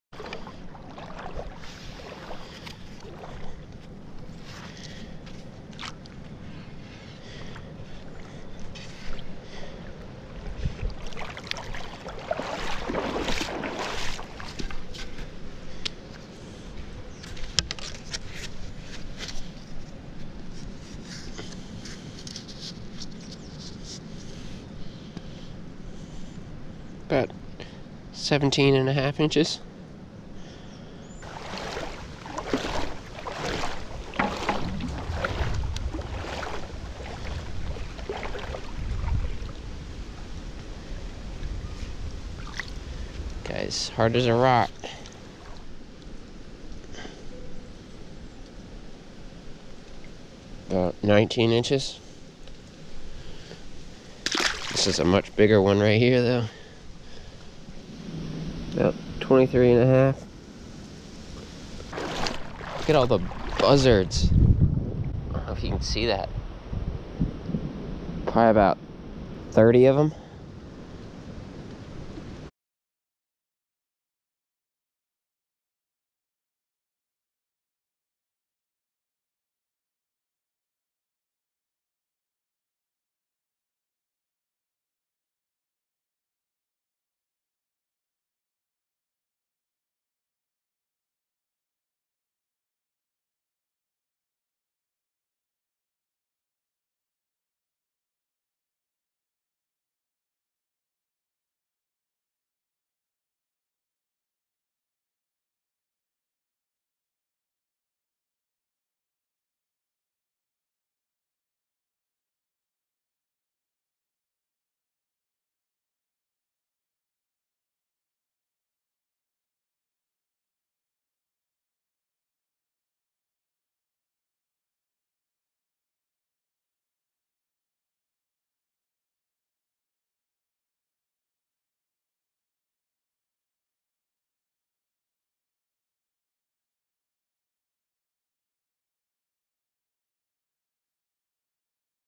Subfreezing Fish Kill Feeding || Speckled Trout
A string of nights with below-freezing temps sadly dooms some remaining speckled trout in the Chesapeake Bay that failed to migrate south for the winter. With the dead fish going to waste, I thought I'd help feed the local waterfowl (and other curious critters). Videos and photos captured with GoPro Hero 7 Silver, Moultrie A-5, and iPhone 8.